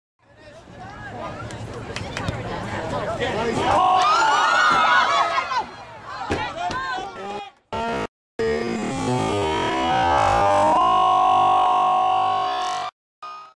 great finger tip save
High school keeper  makes finger tip save to take game to overtime